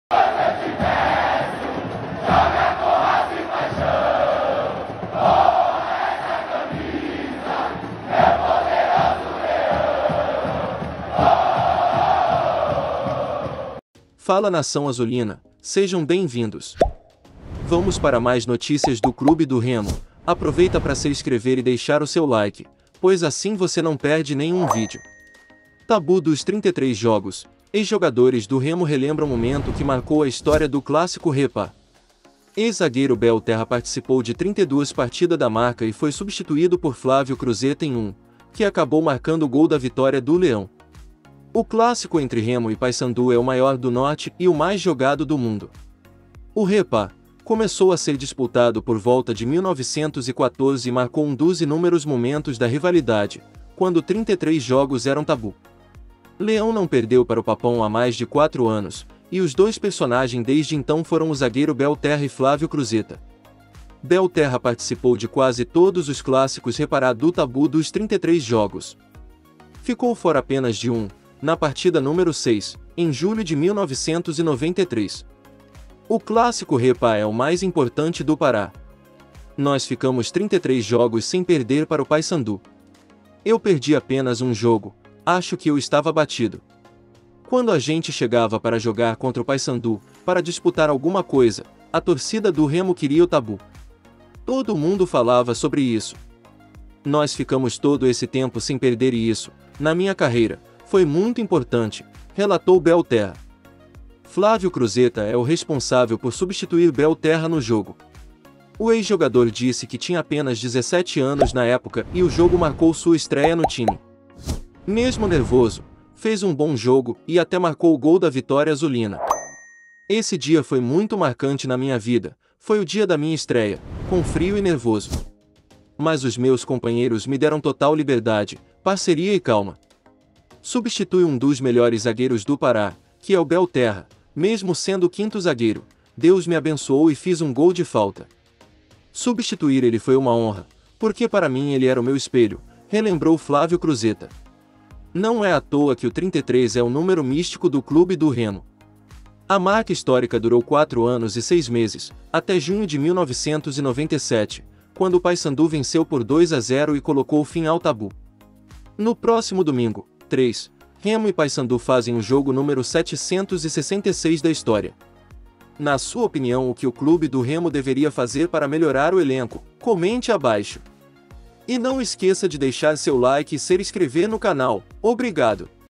RELEMBRE TABU!! TORCIDA VAI A LOUCURA! | NOTÍCIAS CLUBE DO REMO
Tabu dos 33 jogos: ex-jogadores do Remo relembram momento que marcou a história do clássico Re-Pa
Ex-zagueiro Belterra participou de 32 partida da marca e foi substituído por Flávio Cruzeta em um, que acabou marcando o gol da vitória do Leão

O clássico entre Remo e Paysandu é o maior do Norte e o mais jogado do mundo. O Rê-pá começou a ser dispultado por volta de 1914 e marcou um dos inúmeros momentos da rivalidade, quando 33 jogos eram tabu. Leão não perdeu para o papão há mais de quatro anos, e os dois personagem desde então foram o zagueiro Belterra e Flávio Cruzeta.
Belterra participou de quase todos os clássicos Re-Pa do tabu dos 33 jogos. Ficou fora apenas de um, na partida número 6, em julho de 1993.
“O clássico Re-Pa é o mais importante do Pará. Nós ficamos 33 jogos sem perder para o Paysandu. Eu perdi apenas um jogo, acho que eu estava batido. Quando a gente chegava para jogar contra o Paysandu, para disputar alguma coisa, a torcida do Remo queria o tabu. Todo mundo falava sobre isso. Nós ficamos todo esse tempo sem perder e isso, na minha carreira, foi muito importante”, relatou Belterra.
Flávio Cruzeta é o responsável por substituir Belterra no jogo. O ex-jogador disse que tinha apenas 17 anos na época e o jogo marcou sua estreia no time. Mesmo nervoso, fez um bom jogo e até marcou o gol da vitória azulina.
“Esse dia foi muito marcante na minha vida, foi o dia da minha estreia, com frio e nervoso. Mas os meus companheiros me deram total liberdade, parceria e calma. Substitui um dos melhores zagueiros do Pará, que é o Belterra, mesmo sendo o quinto zagueiro, Deus me abençoou e fiz um gol de falta. Substituir ele foi uma honra, porque para mim ele era o meu espelho”, relembrou Flávio Cruzeta.
Não é à toa que o 33 é o número místico do clube do Remo. A marca histórica durou quatro anos e seis meses, até junho de 1997, quando o Paysandu venceu por 2 a 0 e colocou fim ao tabu.
No próximo domingo (3), Remo e Paysandu fazer o jogo número 766 da história.